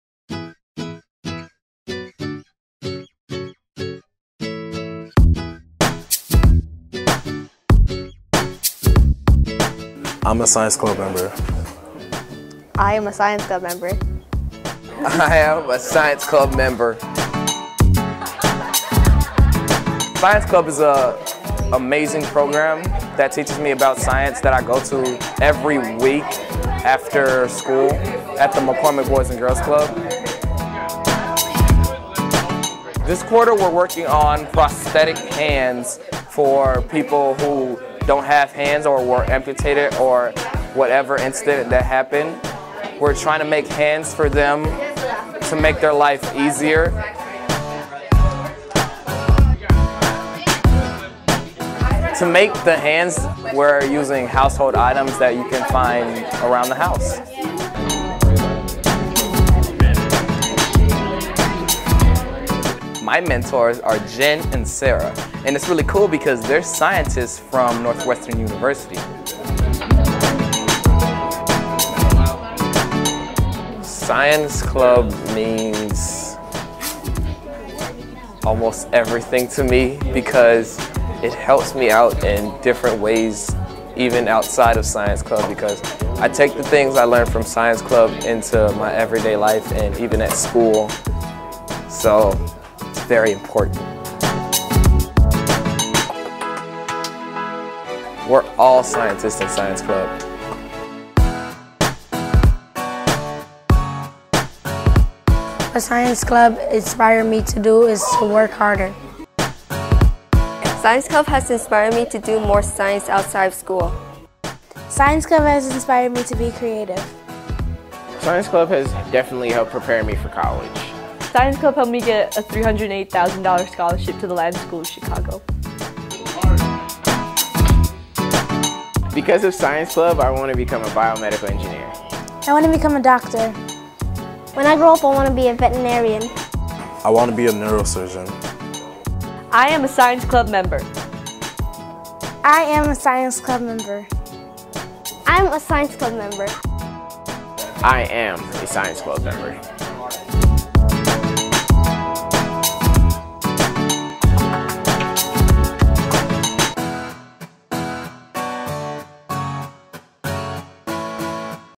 Science Club - Inspiring Chicago Youth
Did you have this much fun learning science in middle school? Science Club offers a new approach to urban science education, designed around long-term mentoring relationships. It integrates community expertise from the Boys & Girls Clubs of Chicago, dedicated teachers from Chicago Public Schools, and engaging scientist mentors from Northwestern University to form a powerful educational team.

On a weekly basis throughout the year, youth work in small groups with Northwestern scientists on fun, engaging curricula designed to foster creativity, critical thinking, writing, and analytic skills. Mentor-youth relationships persist from quarter to quarter and year to year, creating bonds and deeper learning that will last a lifetime.

Mentors benefit tremendously from the experience, too. They learn advanced teaching skills, strategies to design curriculum, and evaluation techniques that prepare them to be engaged scientists and informal science education leaders.

Science Club truly makes a difference:

- Each child in Science Club receives 50 hours of mentor-led, engaging, and challenging science instruction throughout the academic year. In many cases, this doubles the amount of classroom science instruction received in school

- The average student's time in the program is over one year; some students have been in the program for more than four years

- Rigorous evaluation data collected over the last three years at three separate schools show a dramatic increase of 40% in science skills compared to well-matched controls

- Science Club alumni are choosing pre-health career tracks in high school, including nursing, pharmacy, and medicine

- Mentors report significantly higher skills in science communication, teaching, and program development

Visit us online at scienceclub.northwestern.edu

Science Club is an educational research study made possible by a Science Education Partnership Award (SEPA) from the National Institutes of Health (R25OD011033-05 IRB STU13112)